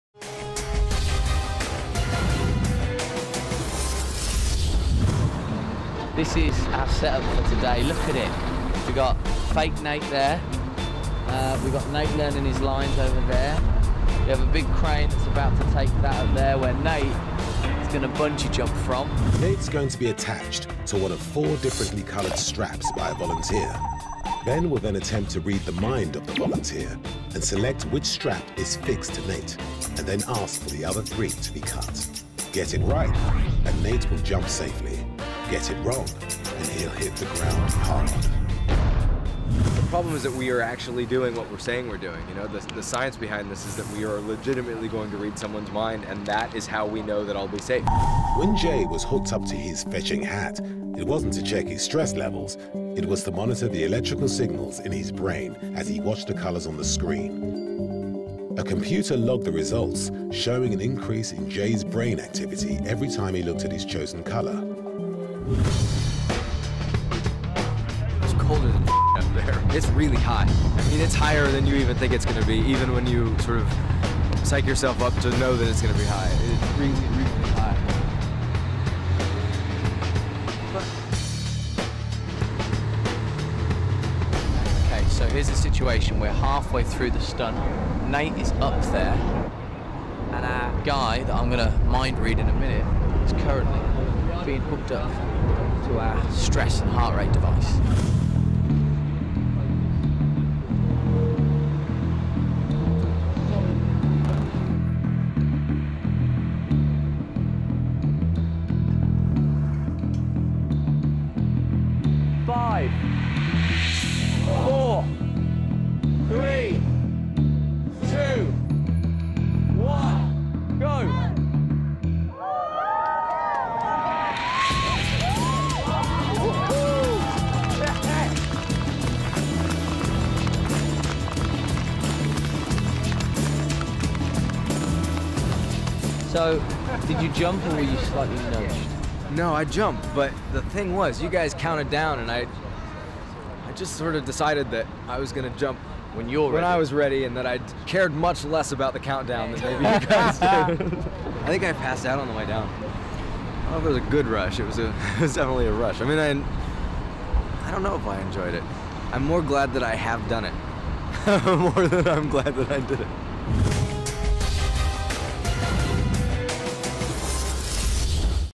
Mind Reading Bungie | Behind The Science
Go behind the scenes to see how the technology used to read minds worked, ensuring Ben didn't have to worry about Nate bungie jumping to his death.